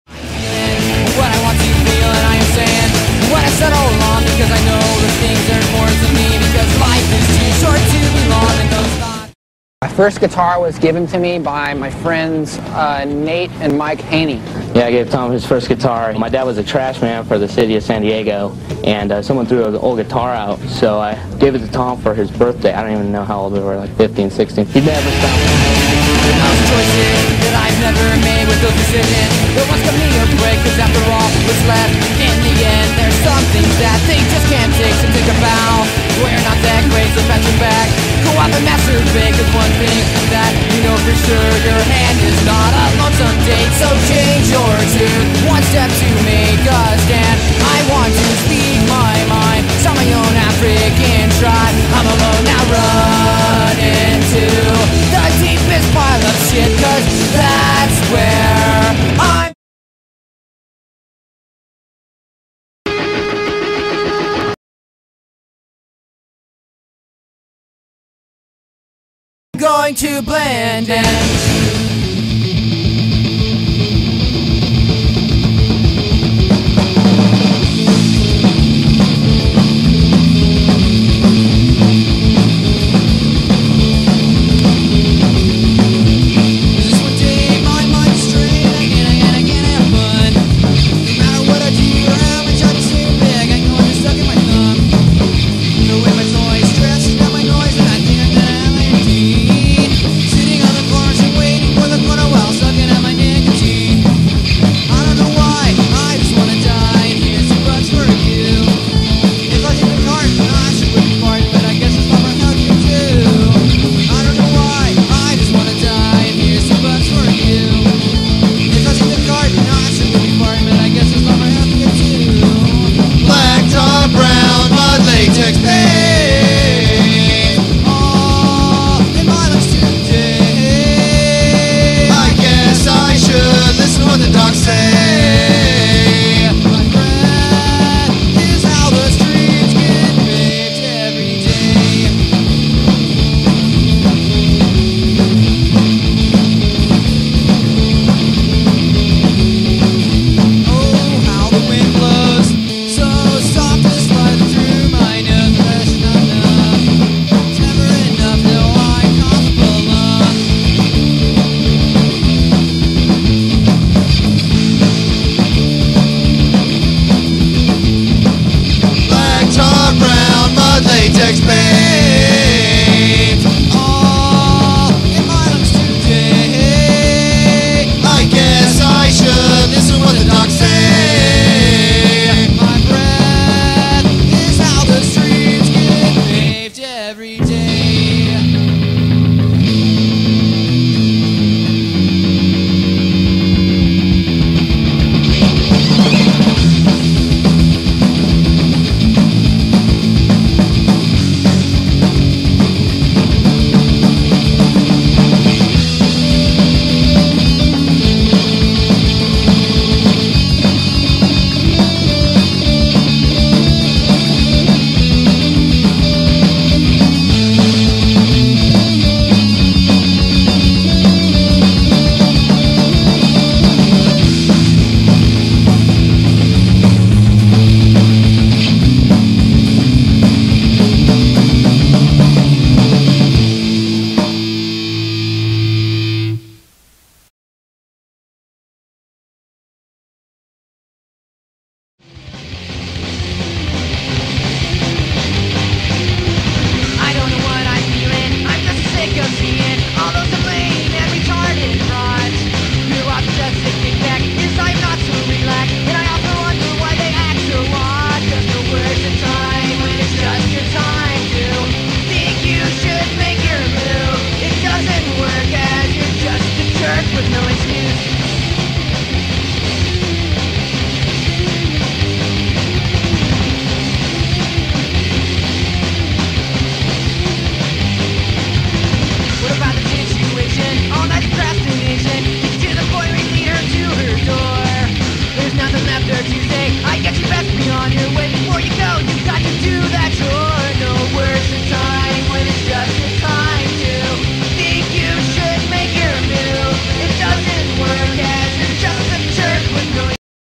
BLINK TOM DELONGE PLAYS THE FIRST GUITAR + rare photo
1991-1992 before become a punk rock star
FUCK NEW BLINK 182!!